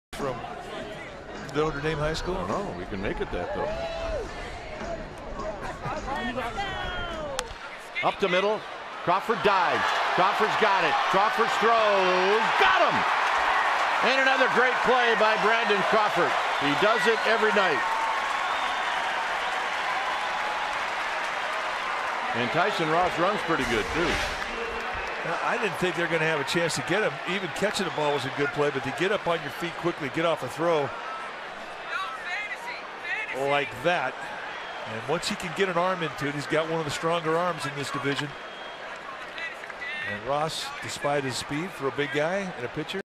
SD@SF: Crawford makes a great stop, fires for out
5/4/15: Brandon Crawford makes a fine stop on a ball up the middle, and throws to first in time to nab Tyson Ross